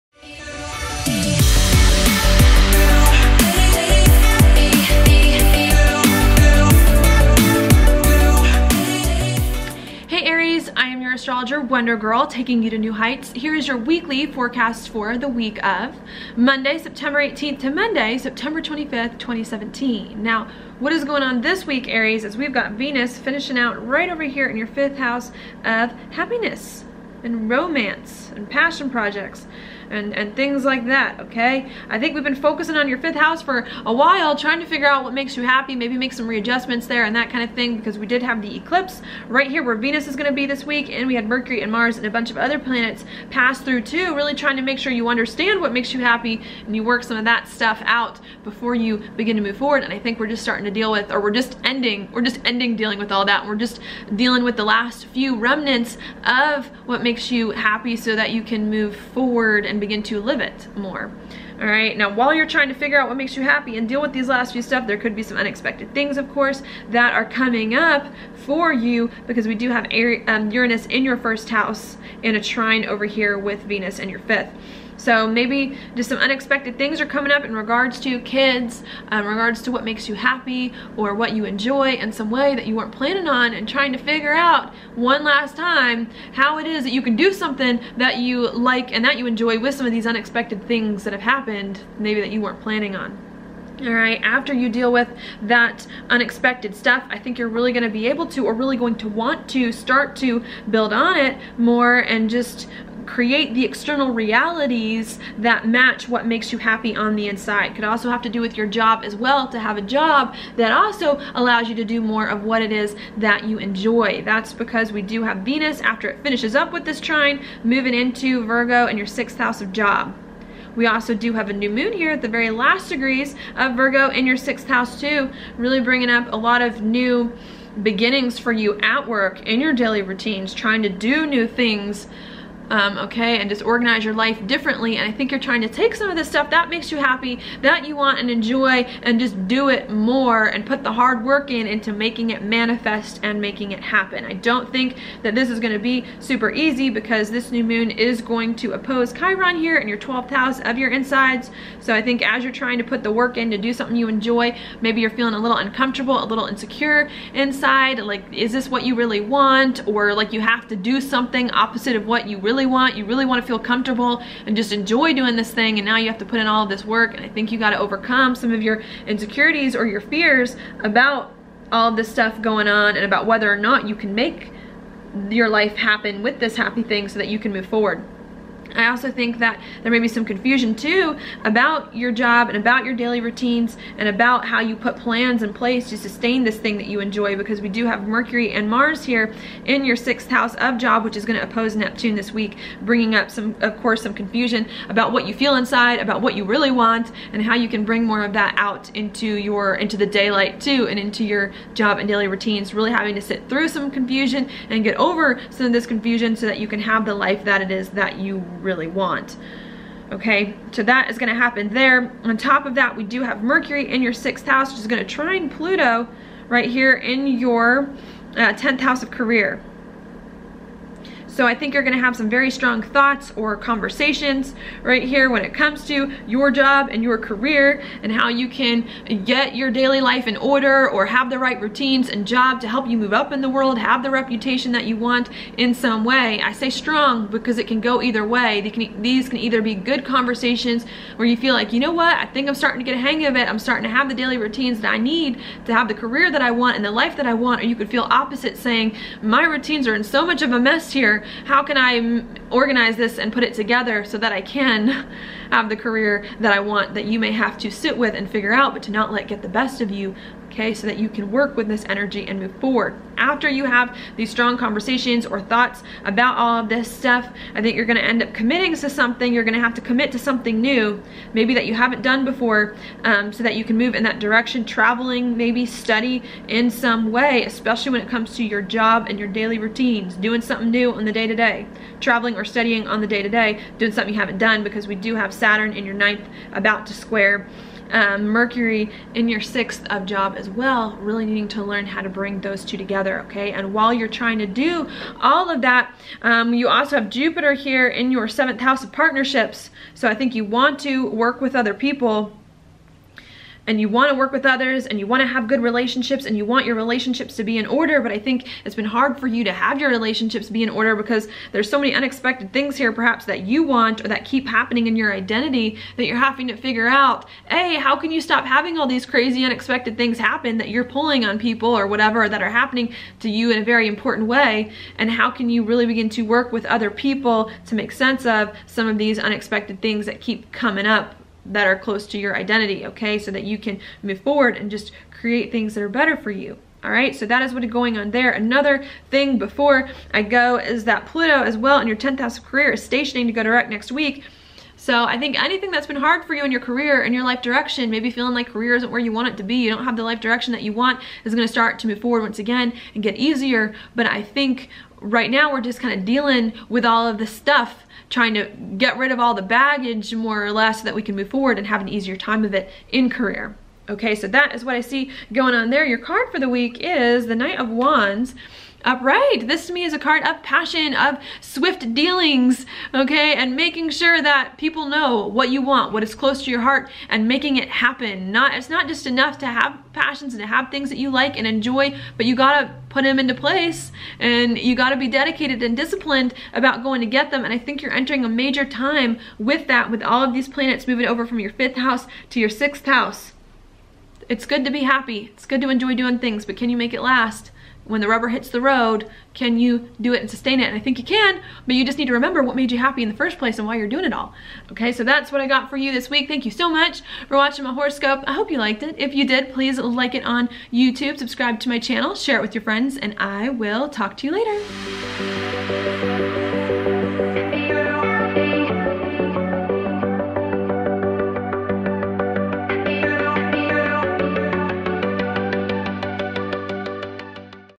Aries 18-25 September 2017 "TRYING NEW THINGS" Weekly Horoscope
Here is your Aries horoscope for the week of Monday, September 18 to Monday, September 25, 2017:

You're trying to create a life doing more of what it is that you love and enjoy. However, it may be harder than you think! Or perhaps more uncomfortable than you want it to be. You could also be very confused on how exactly to do what it is that makes you happy.

Try to stay focused and not feel too bad or insecure about what is going on. You are doing fine, and will continue to do fine if you just stay the course and listen to your intuition.

I think you are trying to change your career or the course of your life so that it aligns more closely with what you want to be doing. Think about that this week, but don't get too caught up in some intense feelings.

The solution to having the life or career you want may require you to do new things at work or in your daily routines in order for you to truly welcome and sustain this thing that you enjoy.